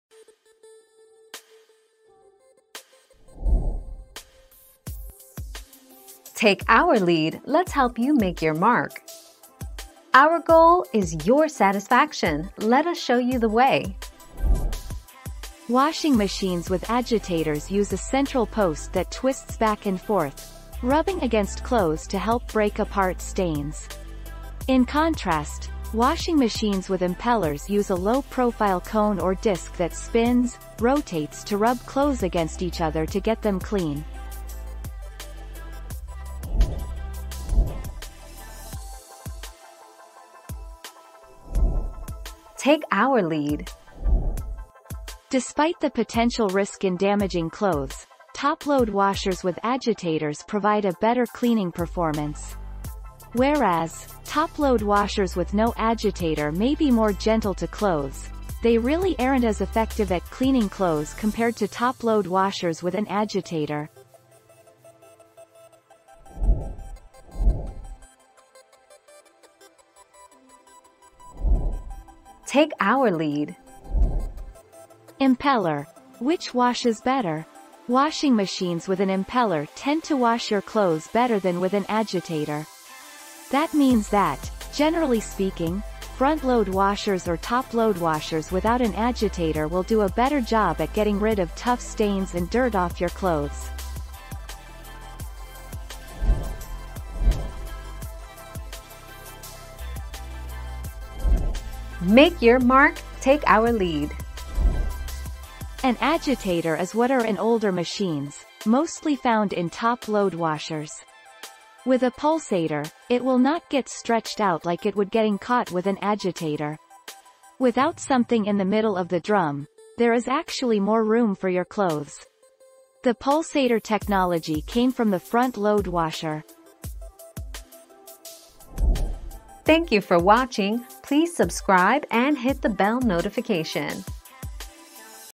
What goes in the washing machine agitator?
00:00 - What goes in the washing machine agitator?
00:38 - Do washers without agitators get clothes clean?
01:08 - Do agitators clean better than impellers?
01:37 - Which is better agitator or pulsator?

Laura S. Harris (2021, July 13.) What goes in the washing machine agitator?
    AskAbout.video/articles/What-goes-in-the-washing-machine-agitator-258352